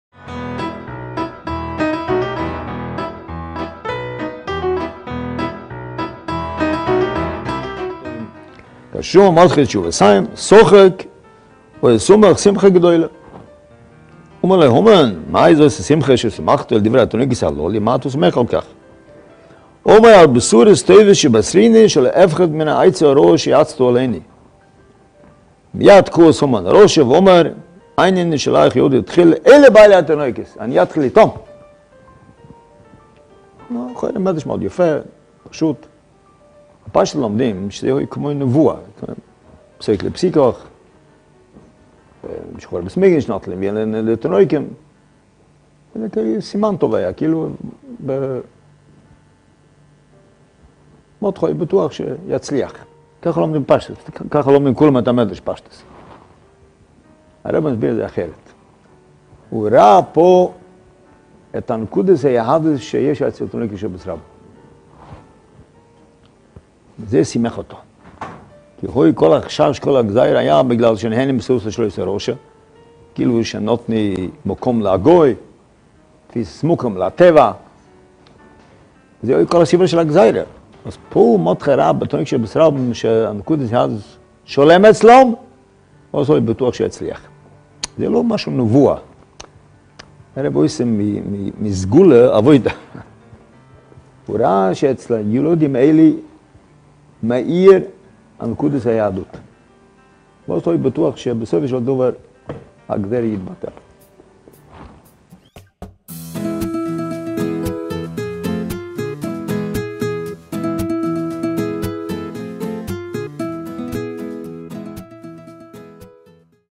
הרה"ג הרב ווכטר שליט"א - פורים, בזכות הילדים!
המעלה של הילדים, מה מרדכי ראה אצלהם,